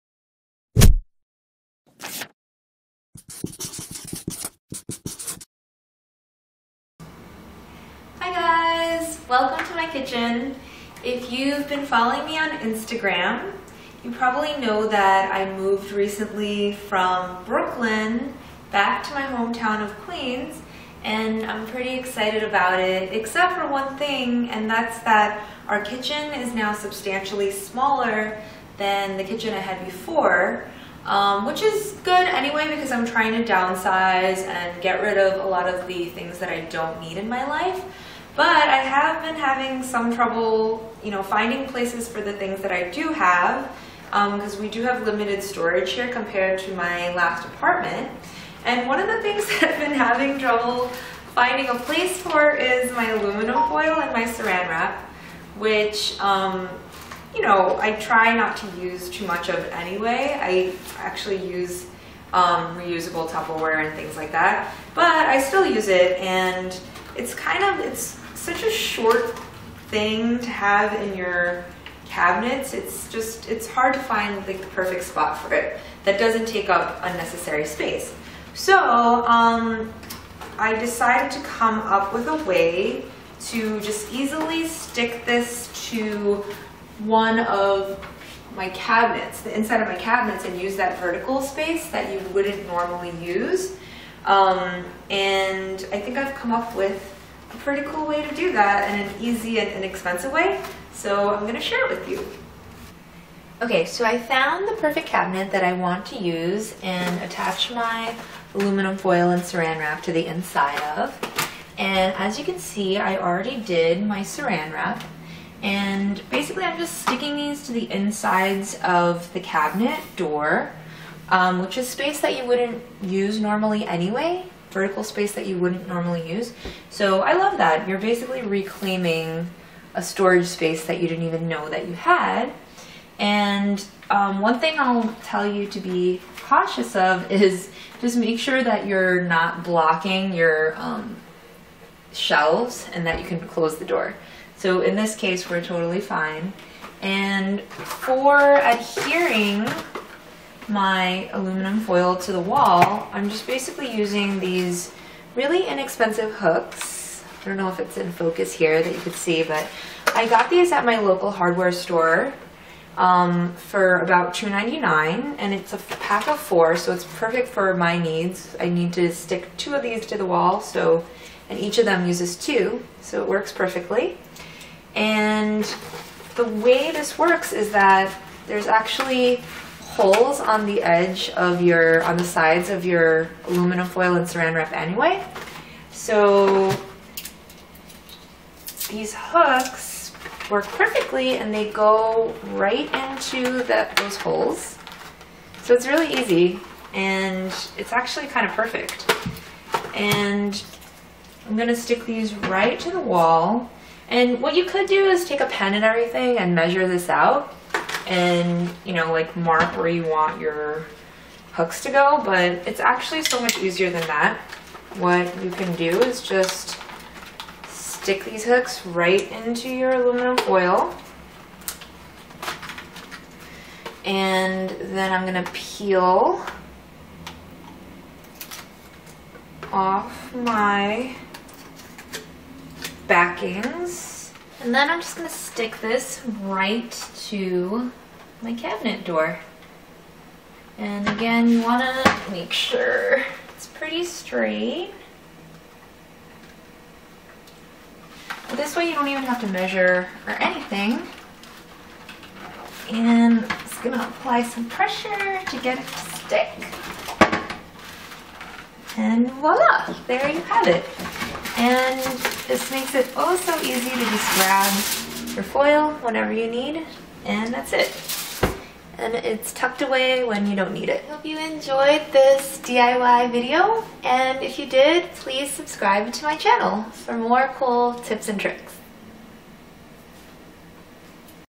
Easy and Clever DIY Way to Store Foil and Saran Wrap in Your Kitchen
Learn how to use inexpensive hooks to install foil and saran wrap on the inside of your kitchen cabinets for easy access.